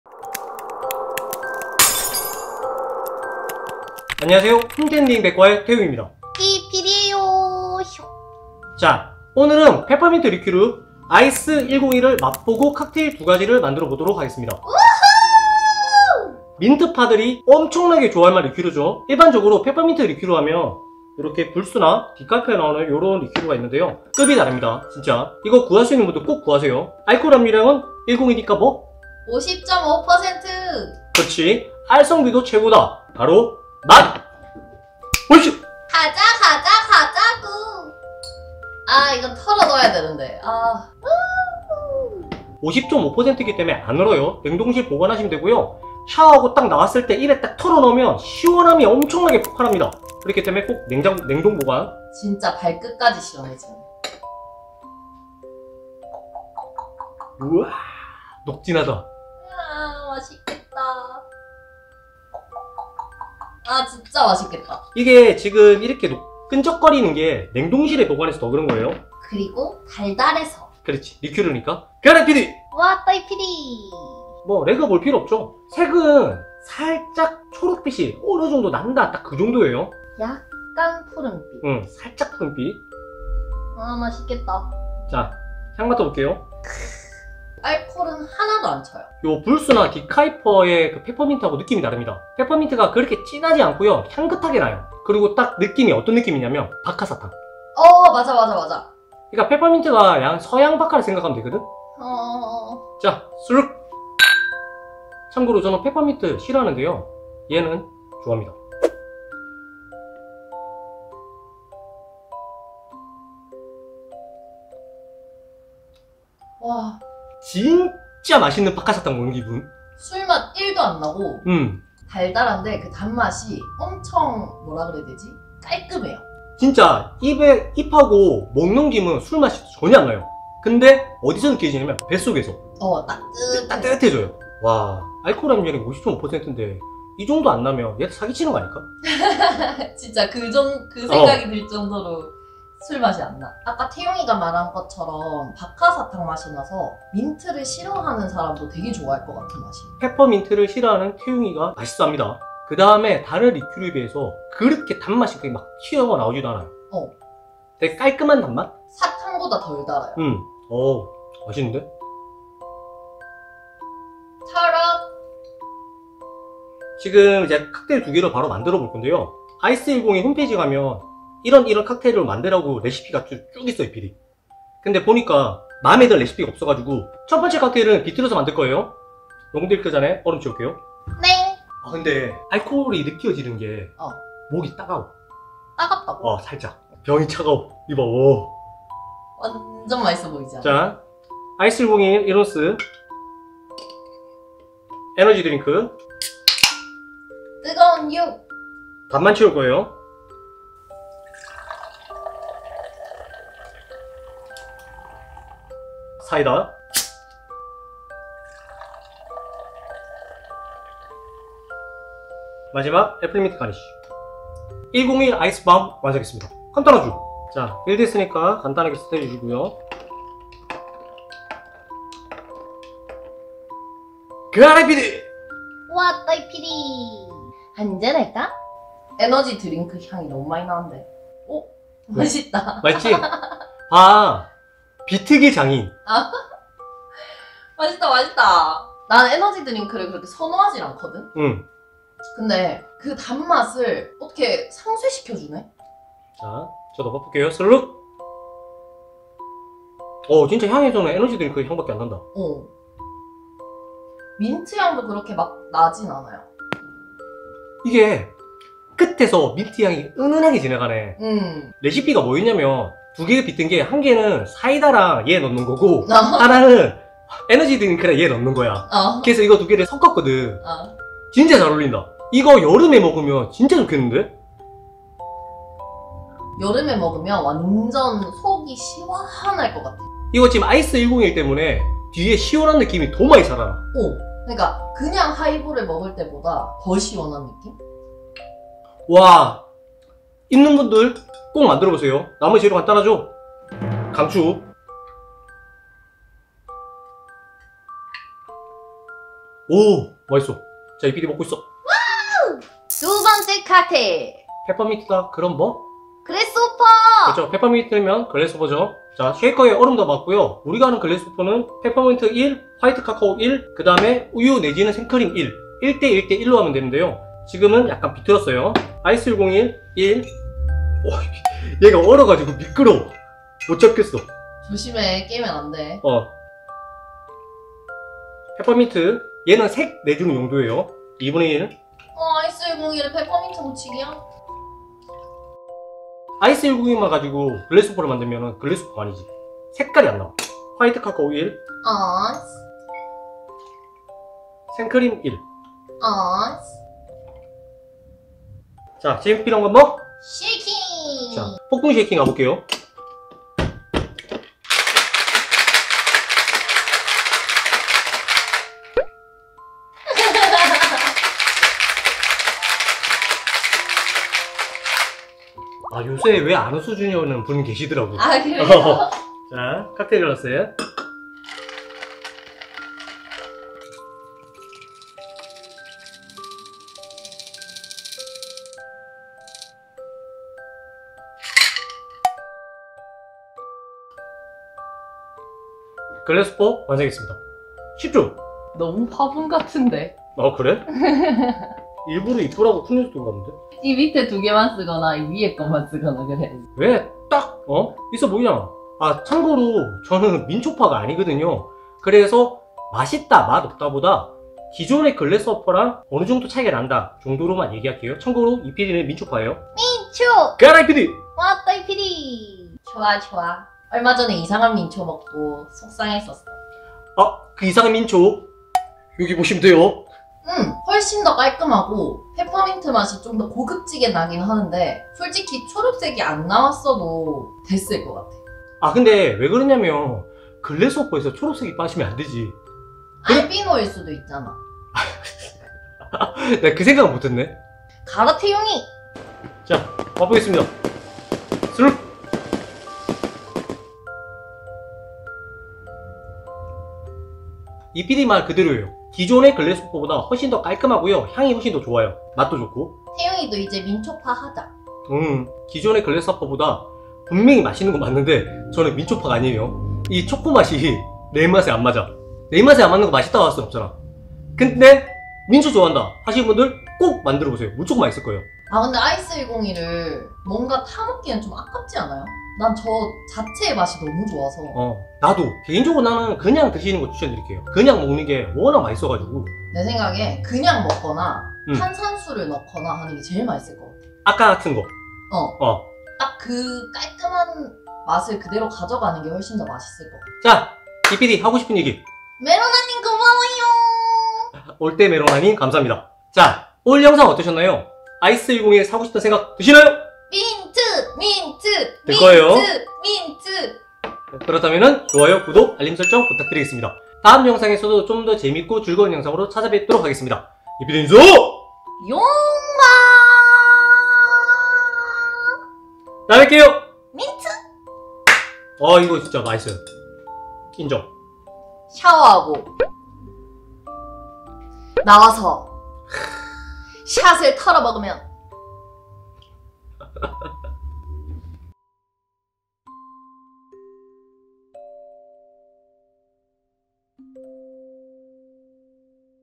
[리큐르]페퍼민트 맛 아이스 101🧊 발끝까지 시원해지는 맛! | 아이스밤과 그래스호퍼
완전완전 맛있는 민트 리큐르 아이스 101🧊
그냥 마셔도 맛있고 얼려 먹어도 맛있고 칵테일로 타도 맛있는 리큐르!!!

[Video Source Support]
!편집하는 여자님 완전 사랑합니다!